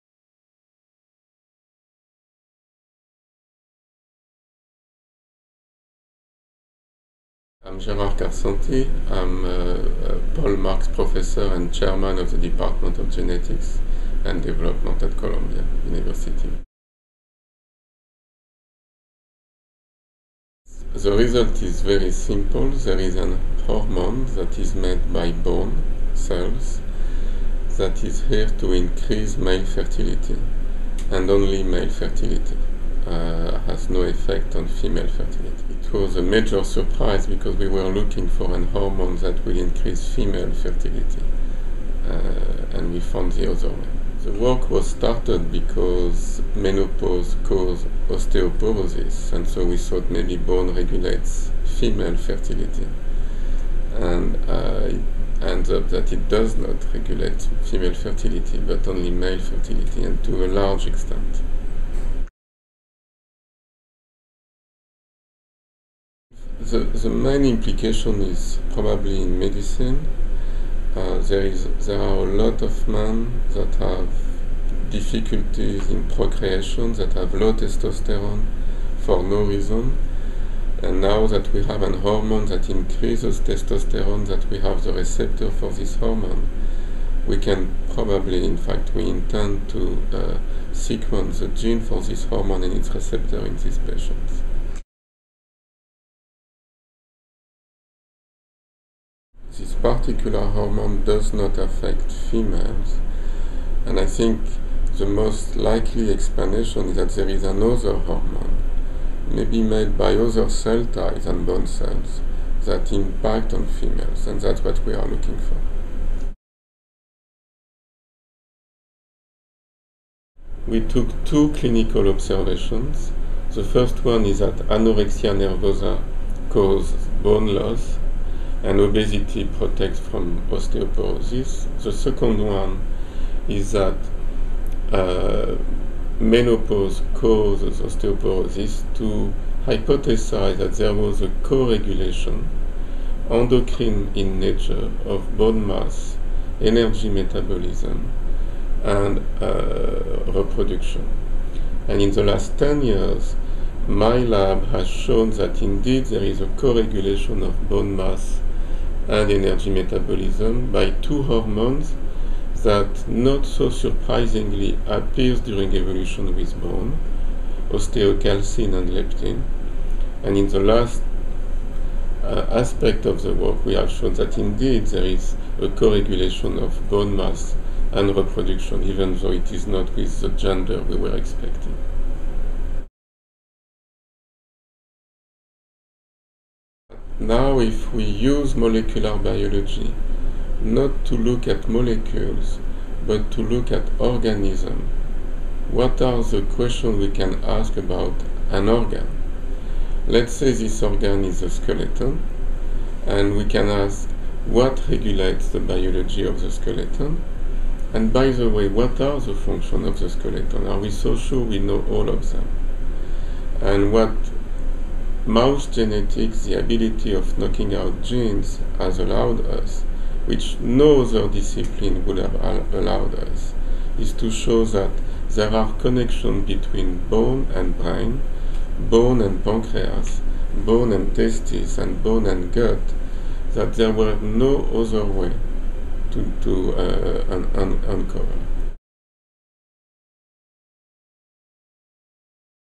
Gerard Karsenty, MD, PhD
Dr. Gerard Karsenty discusses his recent research on bone and its relationship to male fertility.